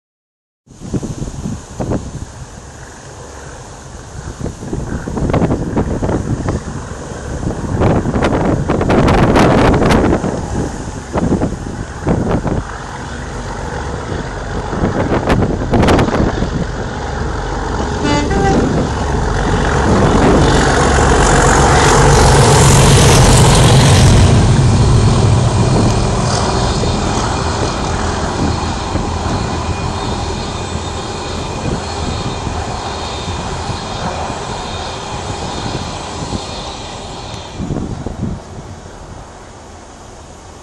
37608+37609 4C03 Winwick Jcn 11/09/12
37608+37609 thrash past Winwick Jcn with 4C03 Washwood Heath - Carlisle. The DRS 37's were on hire to DBS, the 964 tonne train giving them a good workout